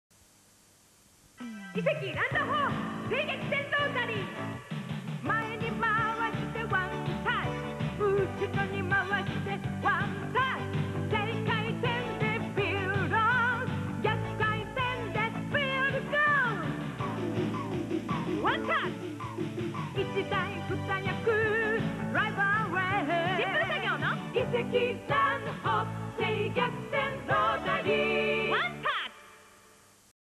1985 井関農機 LAND HOPE 正逆転ロータリー CM JAPAN
１９８５年（昭和６０年）
井関農機　LAND　HOPE　正逆転ロータリー　CM　です

ＶＨＳテープ　３倍モード　での録画になりますので